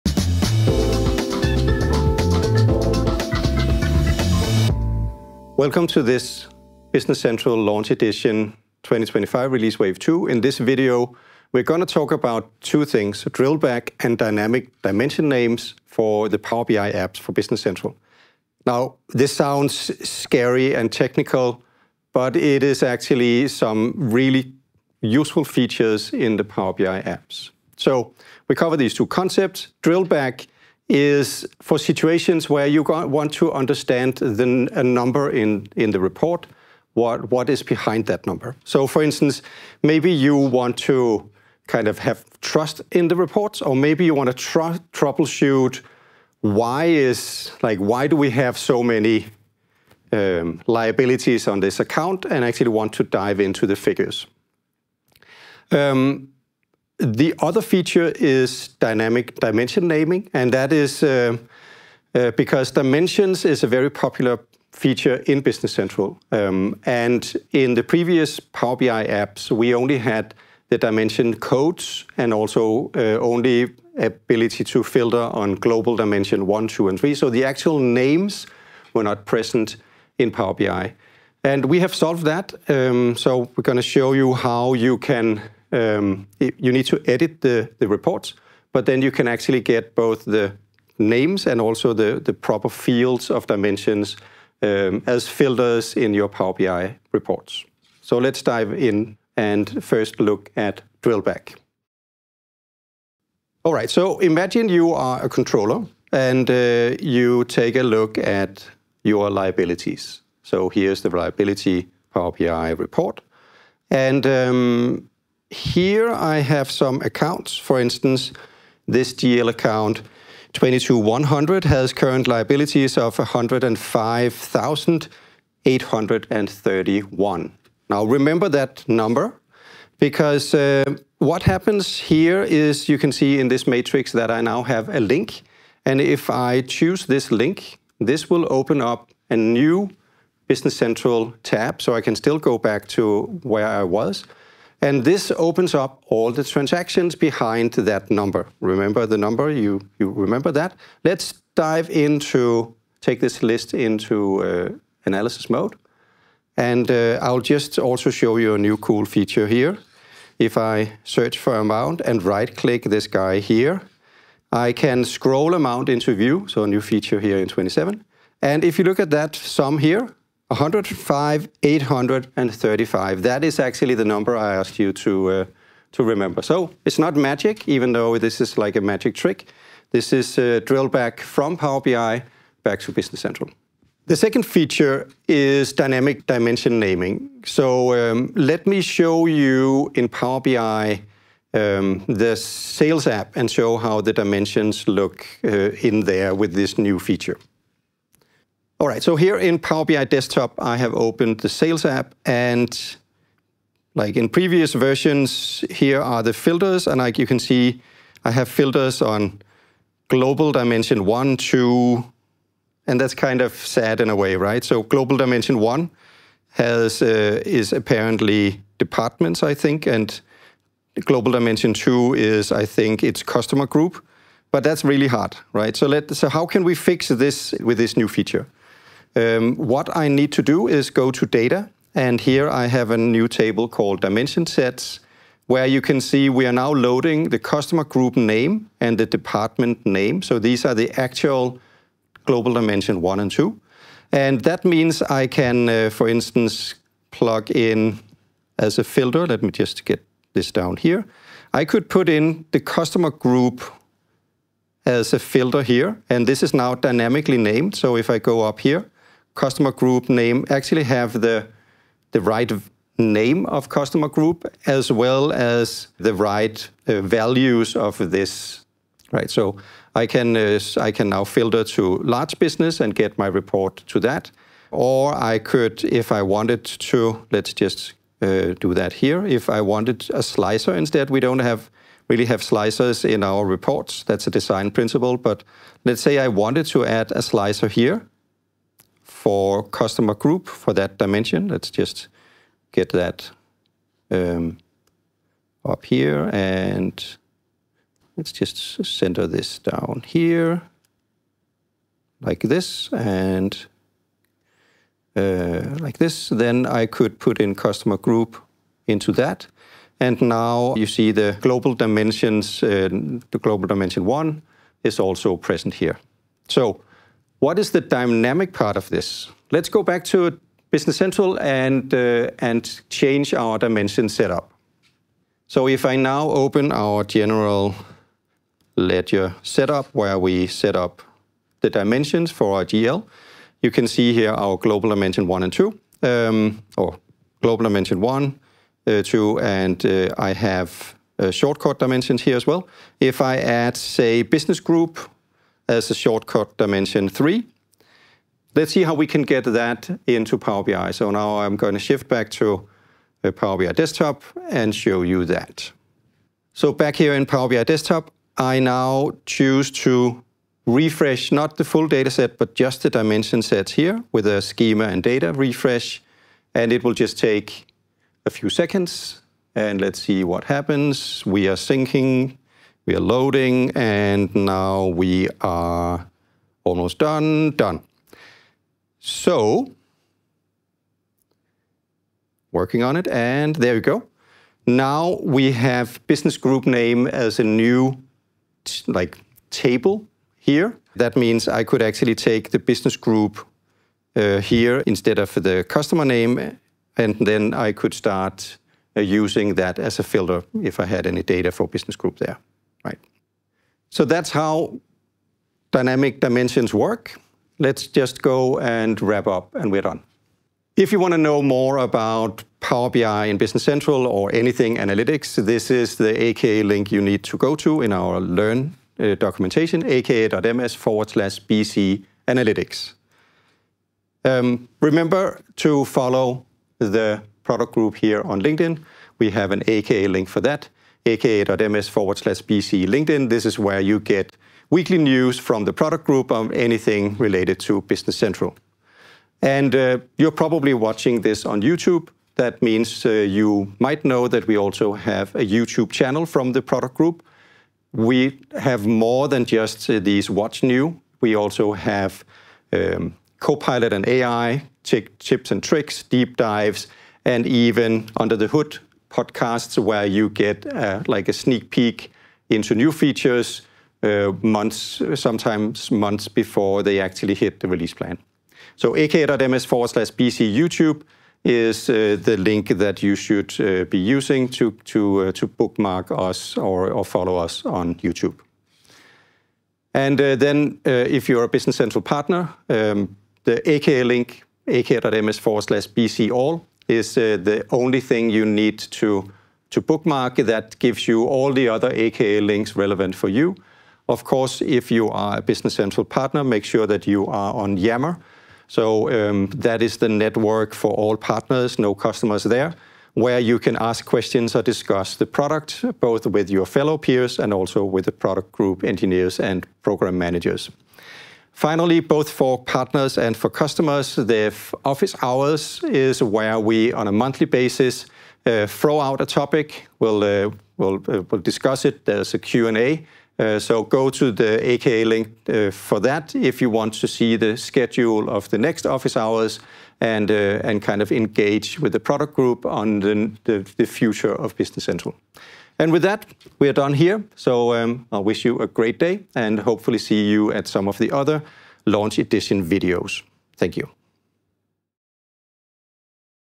What's new: Drill-back and dynamic dimension name support in Power BI apps for Business Central
Drill-back is for situations where you want to dive into the specifics of a number shown in Power BI. Maybe you want to understand how that number is calculated to gain trust in your reporting, or maybe you need to dig into the background of a certain number. Can it really be true that our COGS is that high? Let's investigate.

Dimensions is a very popular analysis feature in Business Central and it has been in the product for many years. With dimensions you have a very dynamic way of defining dimension names and values, both with code fields and with user-friendly names. With dynamic dimension naming in Power BI, we transfer this flexibility to your Business Intelligence reports. This allows you to also analyze your data by dimension when you are in Power BI.

0:28 Why we added drill-back and dynamic dimension naming support to the Business Central Power BI apps
1:39 Demo of drill-back from Power BI to Business Central
3:04 Demo of dynamic dimension naming in the Power BI apps for Business Central
6:50 Learn more about Power BI
7:03 Business Central resources